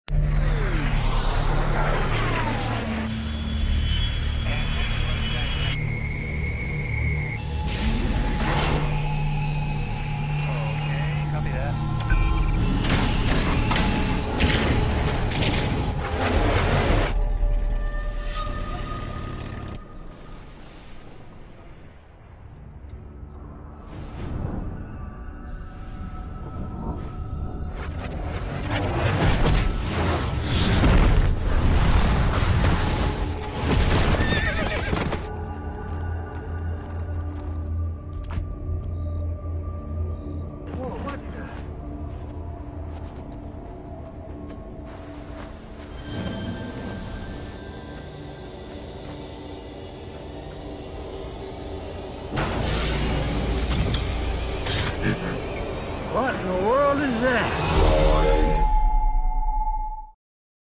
Verizon's DROID ad: Stealth Bomber
Verizon aired this cool high-tech advert ahead of the Droid launch. The carrier said that "the world of doesn't is giving way to a bare-knuckle bucket of does."